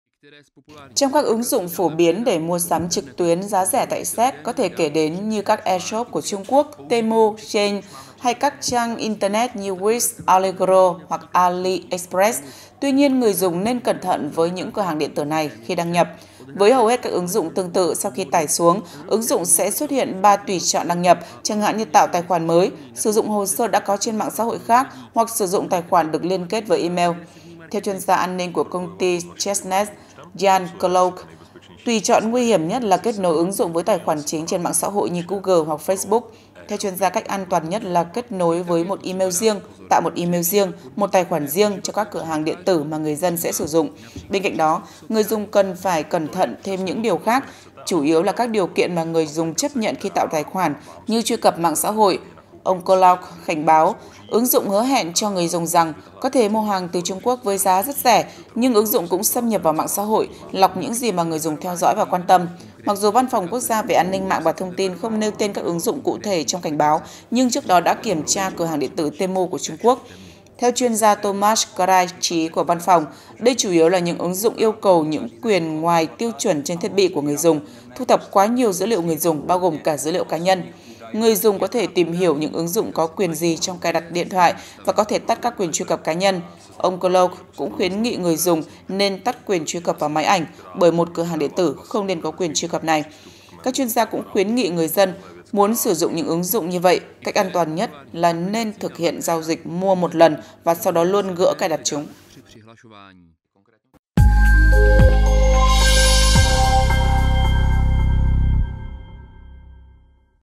Cảnh báo cửa hàng e-shop thu thập thông tin cá nhân của người dùng | TamdaMedia
Theo cảnh báo của Văn phòng Quốc gia về An ninh mạng và thông tin, một số cửa hàng trực tuyến cung cấp dịch vụ mua sắm giá rẻ có thể thu thập lượng dữ liệu quá mức, bao gồm cả dữ liệu cá nhân, riêng tư của người dùng.